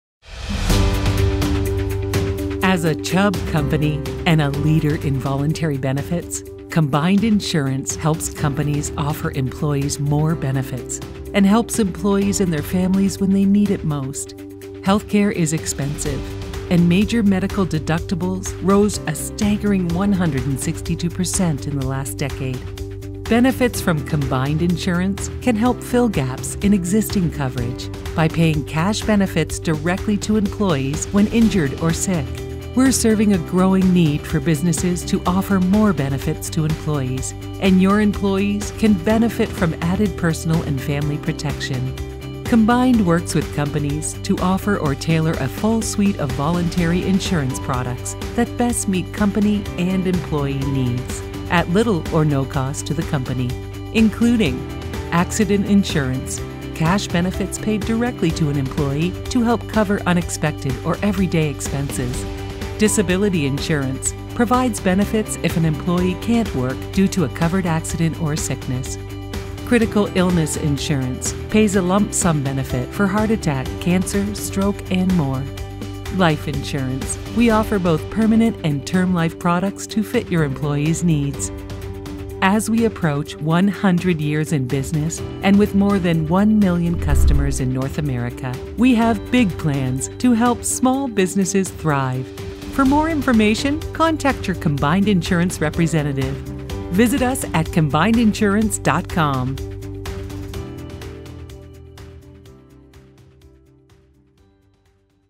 Why offer Voluntary Benefits to your Employees? Find out w/Combined Insurance.
See how Combined Insurance helps large and SMALL businesses offer their employees more benefits. And how we help employees and their families, when they need it most.